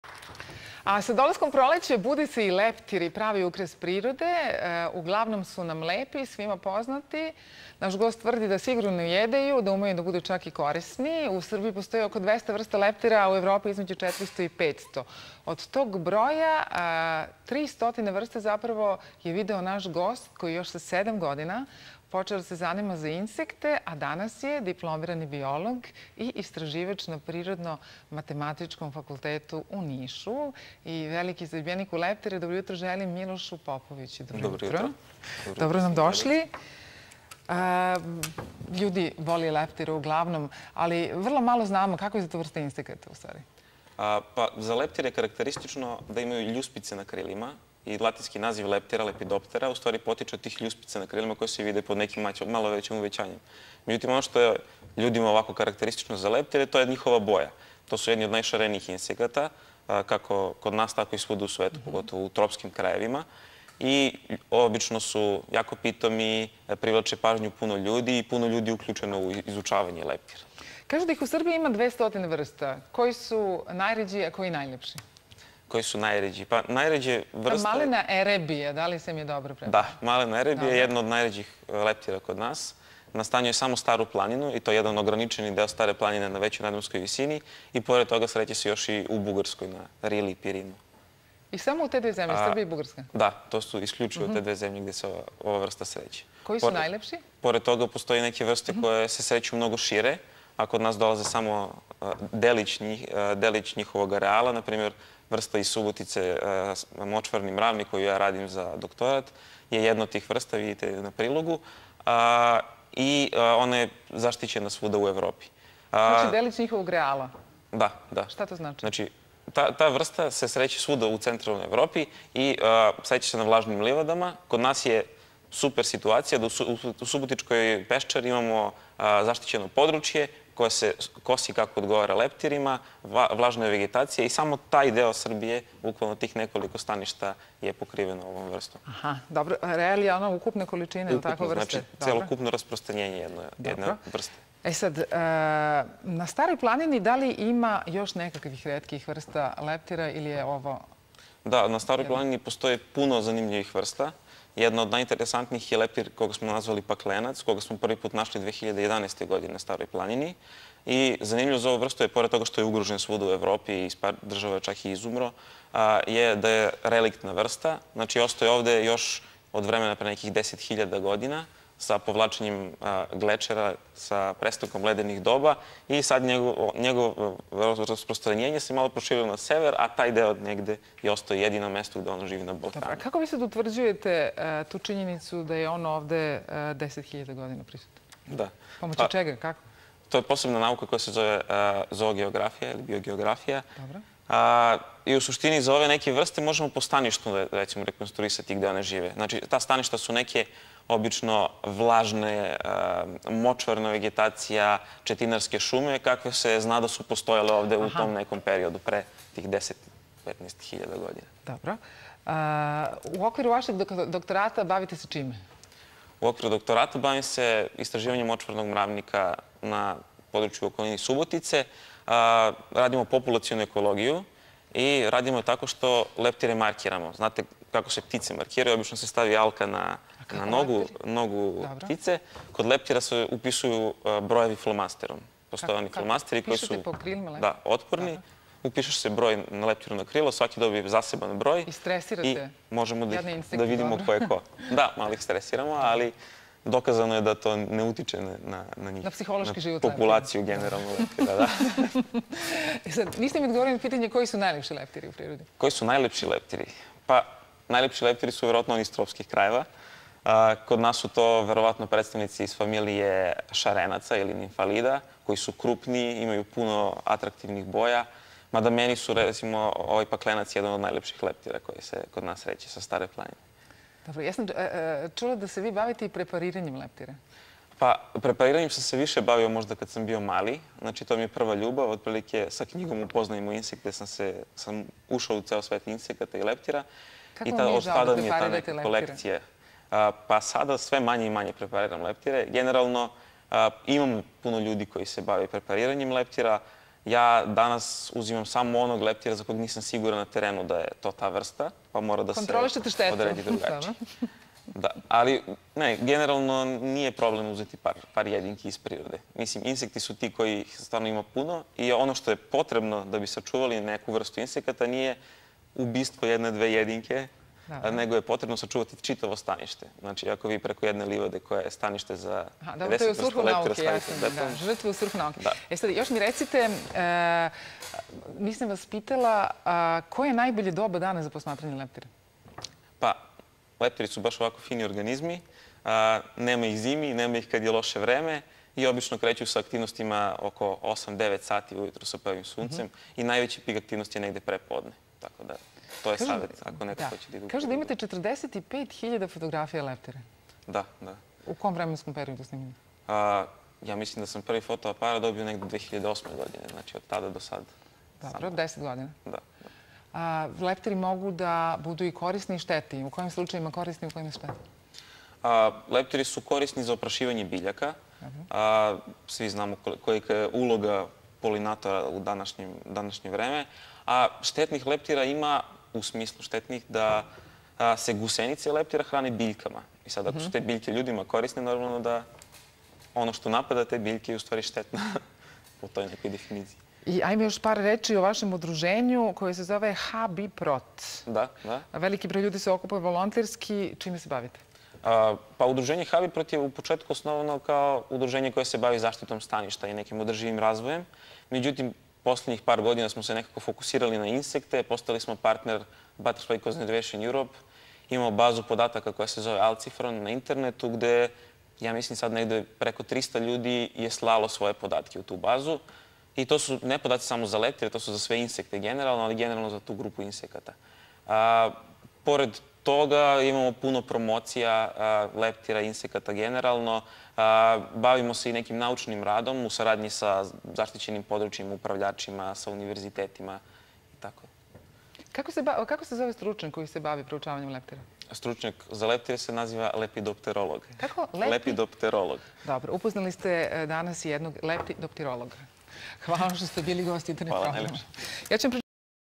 Šarena krila
Leptiri su lepi, svima poznati, ne ujedaju i korisni su. U Srbiji ima oko 200 vrsta leptira, a u Evropi između 400 i 500. U goste nam dolazi najbolji poznavalac leptira, Miloš Popović.